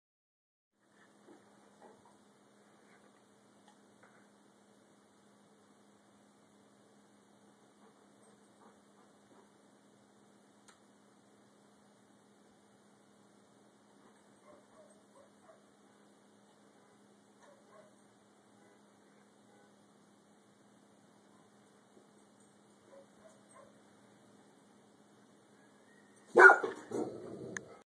EricBitesTheWall.3gp
Eric is a pretty stupid dog. Here he is, thinking his shadow is another dog in the house...a solid black, two-dimensional dog. So, being the bad-ass he is, he bites the "other dog." Idiot...